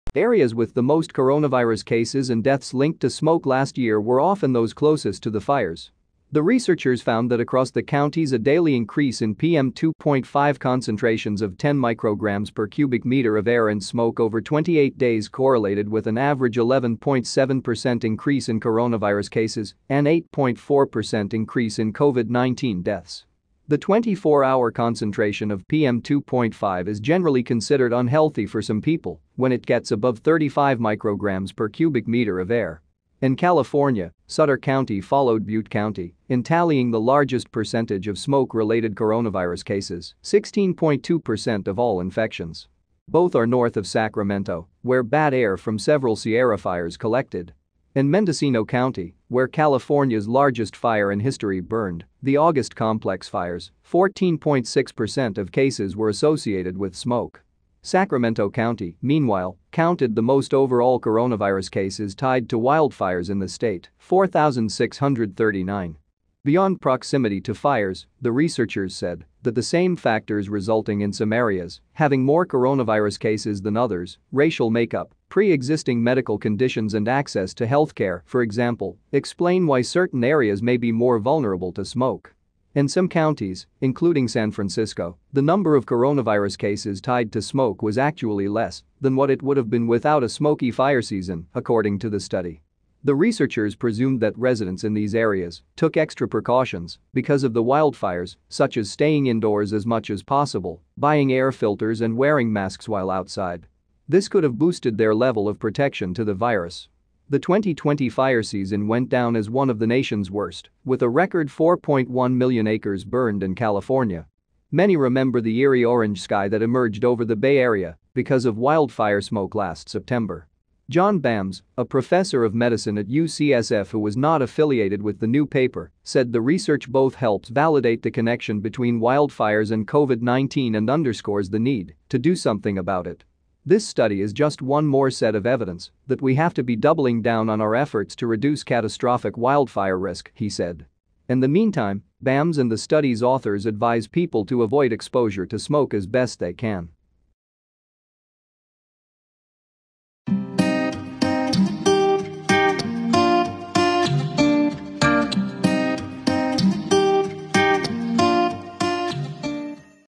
Is wildfire smoke linked to coronavisus cases
Areas with the most coronavirus cases and deaths linked to smoke last year were often those closest to the fires. The researchers found that across the counties a daily increase in PM 2.5 concentrations of 10 micrograms per cubic meter of air in smoke over 28 days correlated with an average 11.7% increase in coronavirus cases and 8.4% increase in COVID-19 deaths. The 24-hour concentration of PM 2.5 is generally considered unhealthy for some people when it gets above 35 micrograms per cubic meter of air. In California, Sutter County followed Butte County in tallying the largest percentage of smoke-related coronavirus cases: 16.2% of all infections. Both are north of Sacramento, where bad air from several Sierra fires collected. In Mendocino County, where California’s largest fire in history burned, the August Complex fires, 14.6% of cases were associated with smoke. Sacramento County, meanwhile, counted the most overall coronavirus cases tied to wildfires in the state: 4,639. Beyond proximity to fires, the researchers said that the same factors resulting in some areas having more coronavirus cases than others — racial makeup, pre-existing medical conditions and access to health care, for example — explain why certain areas may be more vulnerable to smoke. In some counties, including San Francisco, the number of coronavirus cases tied to smoke was actually less than what it would have been without a smoky fire season, according to the study. The researchers presumed that residents in these areas took extra precautions because of the wildfires, such as staying indoors as much as possible, buying air filters and wearing masks while outside. This could have boosted their level of protection to the virus. The 2020 fire season went down as one of the nation’s worst, with a record 4.1 million acres burned in California. Many remember the eerie orange sky that emerged over the Bay Area because of wildfire smoke last September. John Balmes, a professor of medicine at UCSF who was not affiliated with the new paper, said the research both helps validate the connection between wildfires and COVID-19 and underscores the need to do something about it. “This study is just one more set of evidence that we have to be doubling down on our efforts to reduce catastrophic wildfire risk,” he said. In the meantime, Balmes and the study’s authors advise people to avoid exposure to smoke as best they can.